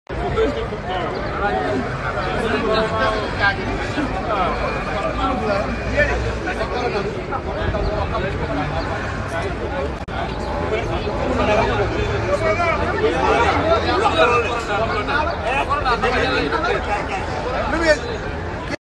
Les derniers moments de Thione Seck et son fils Wally Seck.
Necrologie, Thione Ballago Seck n’est plus 😭 toutes nos condoléances à Wally Seck et toute la famille .